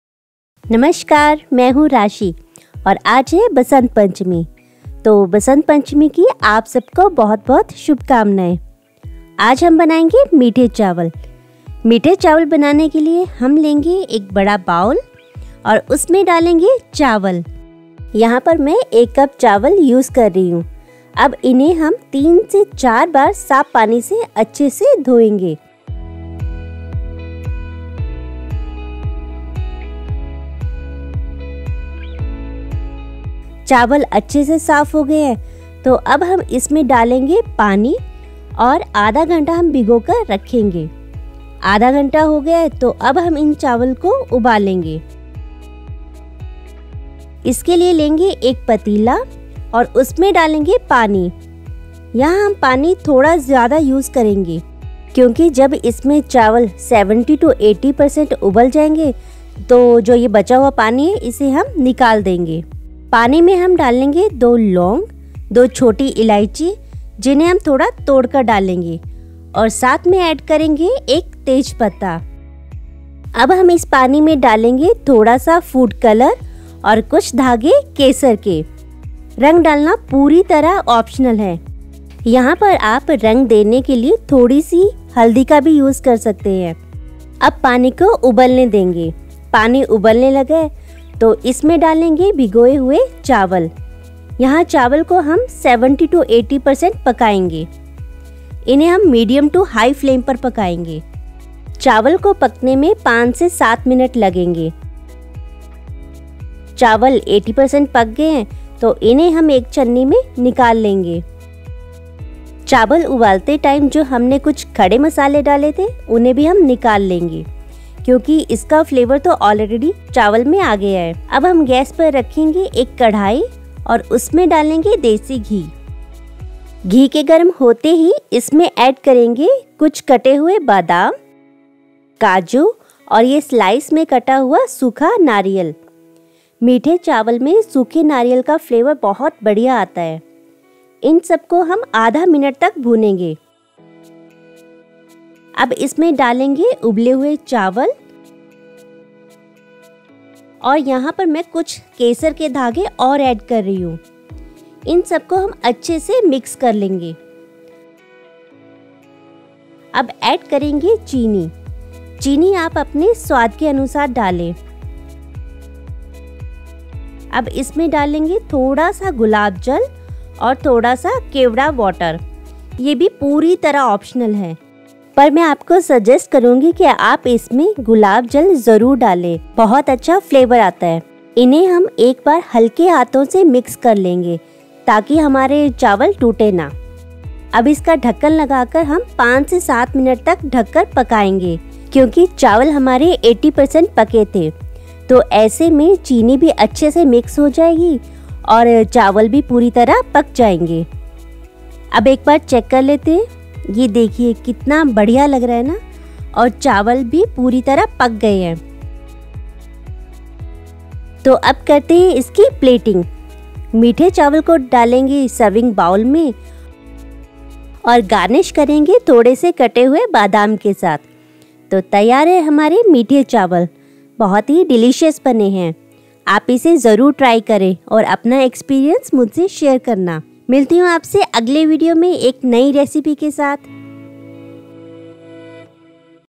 इस बसंत पंचमी में बनाए मीठे चावल | Meethe Chawal | Zarda Pulao Recipe
Aj basant panchami mei banane ja rahi hoon meethe chawal ki recipe, bade hi majedar bante hain or banana bhi asan hai to ap ise try kre or mujhse apna experience share jrur karein.

Ingredients :
Rice - 1cup
Cloves - 2
Cardamom - 2
Bay Leaves - 1 to 2
Food Color - a pinch
Kesar  - 20 to 25 threads
Sugar - according to taste
Ghee - 3 to 4 tbsp
Rose Water - 1 tbsp (Optional)
Kevda Water - 1 tbsp (Optional)

Social media par follow jrur krein